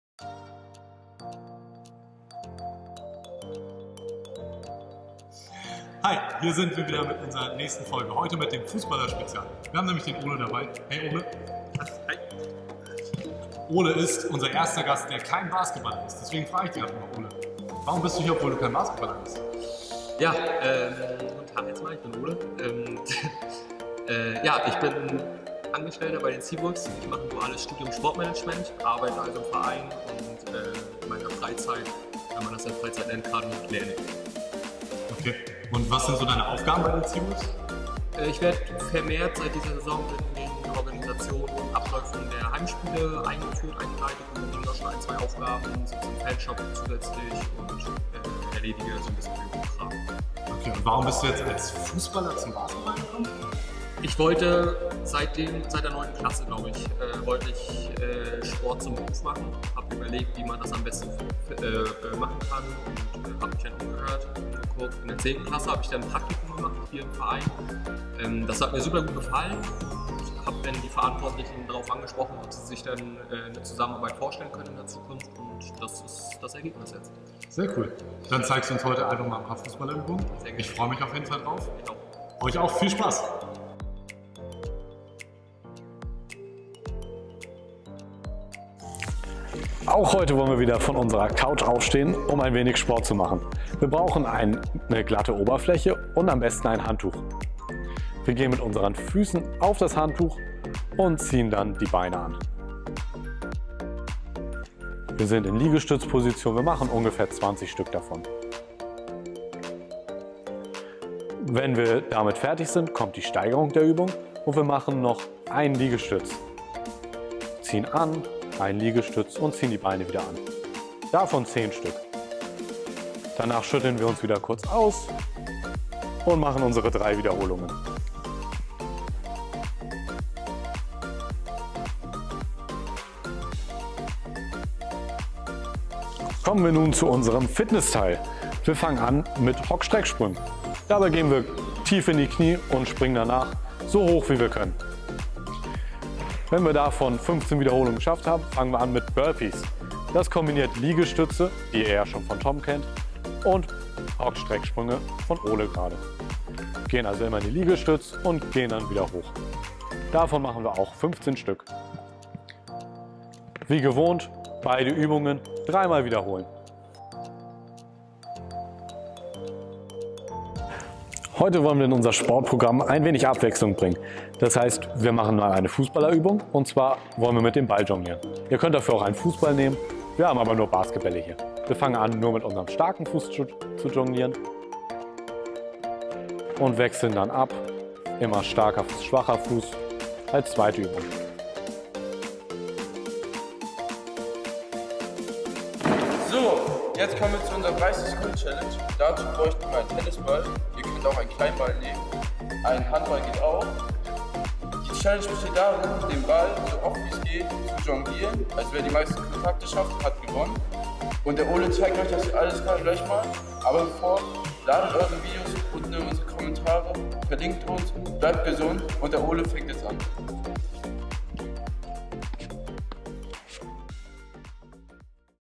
Tägliche EBC Sportchallenge Tag 8 | Workout mit Ole Voigt
Es ist wieder Zeit Hier kommt die neue Ausgabe der „Täglichen EBC Sportchallenge“ präsentiert von der OstseeSparkasse Rostock. Ihr könnt die 7. Sendung ab sofort auf den Social Media Plattformen des EBC Rostock (Facebook und Youtube) abrufen.

In der achten Episode stellt das Moderatoren-Duo Konstantin Brodmeier & Peter Sell den ROSTOCK SEAWOLVES Auszubildenden, Ole Voigt den Zuschauern vor. Ole erzählt über seine Hobby Fußball, sein Sportmanagement Studium und seine Aufgabenfeld bei den "Wölfen". Selbstverständlich hat er auch sein ganz spezielles Workout dabei.

Viel Spaß beim Gucken und Mitmachen.

Nicht vergessen – Videos von der 30. Sekunden Challenge unter dem Facebookbeitrag mit dem ebcchallenge posten und Rekordhalter werden!